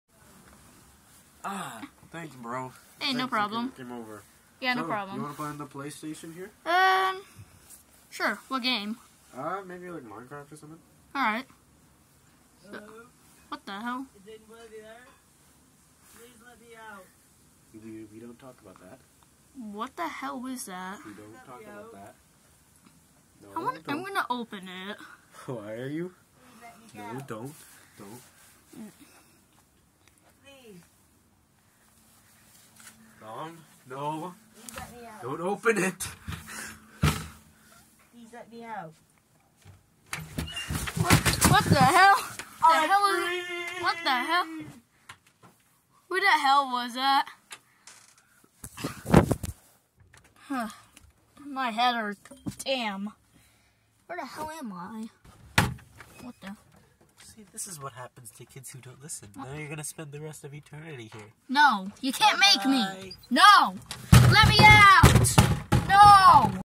The kids in the basement
Idiot boy gets kidnapped by friend (total L)